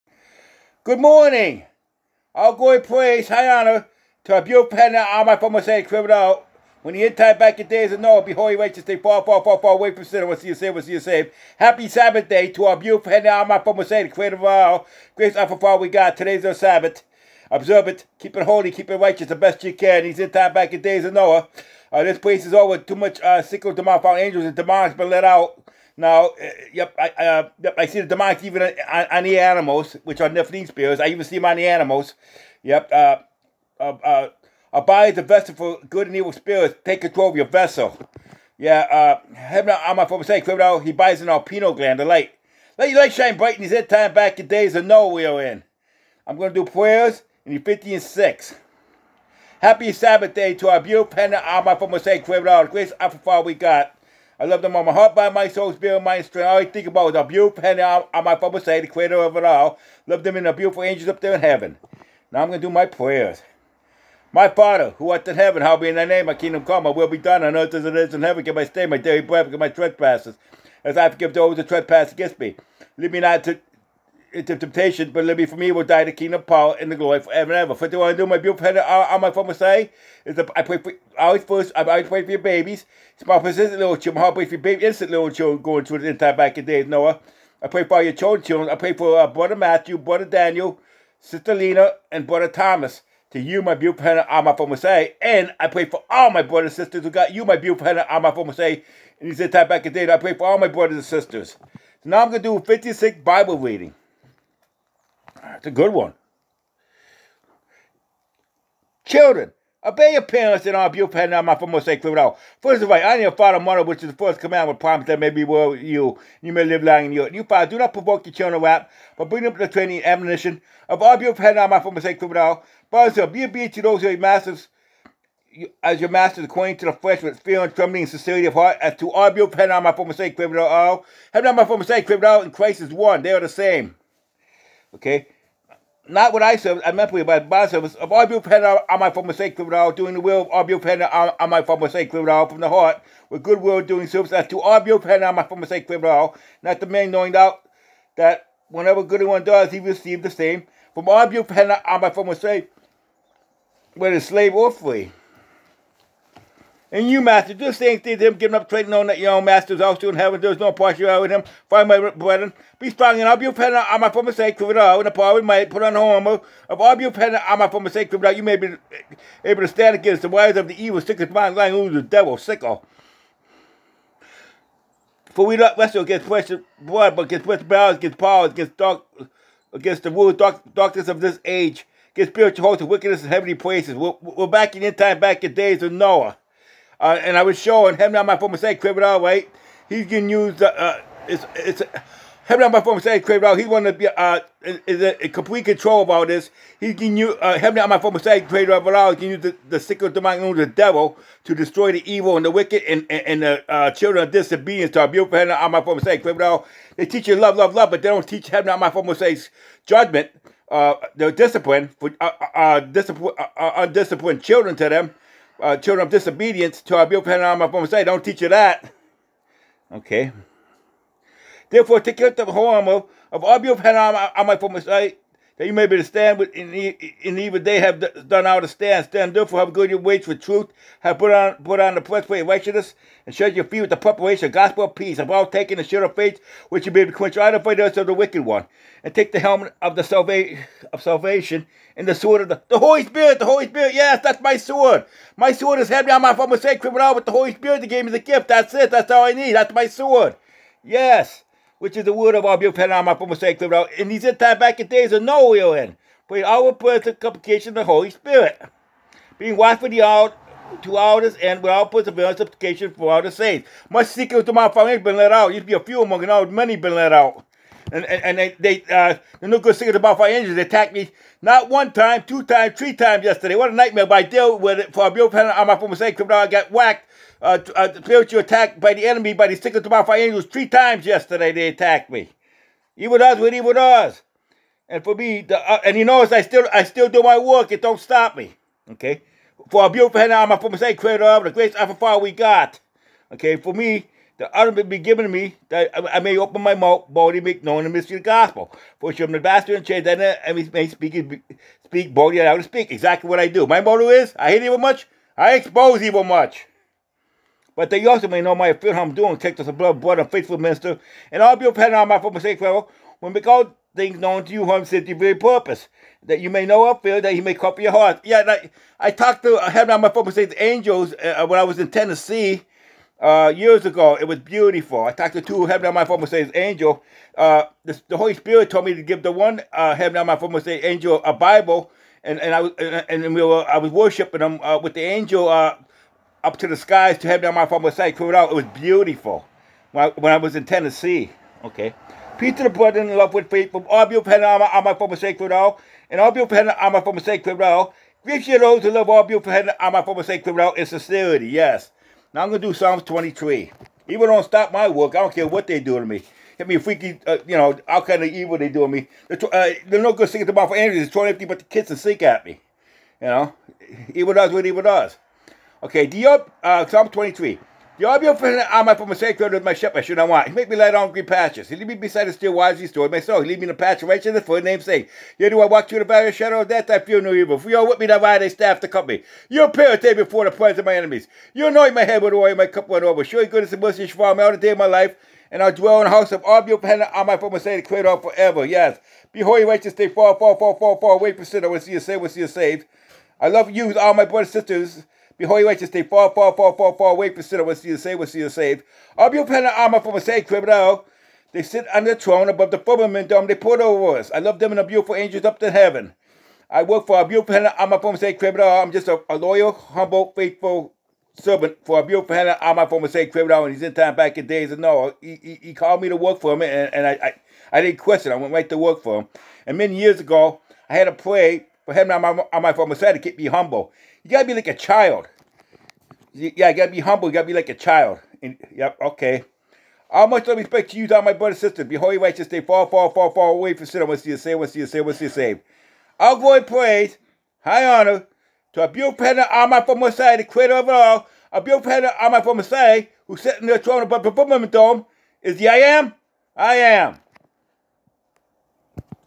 Ephesians6,Psalm23All glory praise high honor to our beautiful Heavenly Father AlmightyThe Most High
All glory,praise high honor to our beautiful Heavenly Father Almighty The Most High The Creator of it Al!!! they is the I am I am Today is our beautiful Heavenly Father Almighty The Most Highs Sabbath!!! keep it Holy and Rightous and stay far, far ,far away from all sinning the best you can . i want to see you saved!!! we our back in the end times back in days of Noah!!! last time was flood , water this time this fake holographic fake reality realm is reserved for Fireeeeeeeeeeeee, lake of fire for the evil and the wicked and for the children of disobedience to our beautiful Heavenly Father Almighty The Most High!!! All much love and respect to all my Brothers and Sisters !!! .,the no good sickos demonic fallen angels spiritual attacked my Spirit!!! 3 times yesterday evil does what evil does !!! ...beautiful Heavenly Father Almighty The Most High created evil for evil and for the days of evil i been shown!!! years ago!!!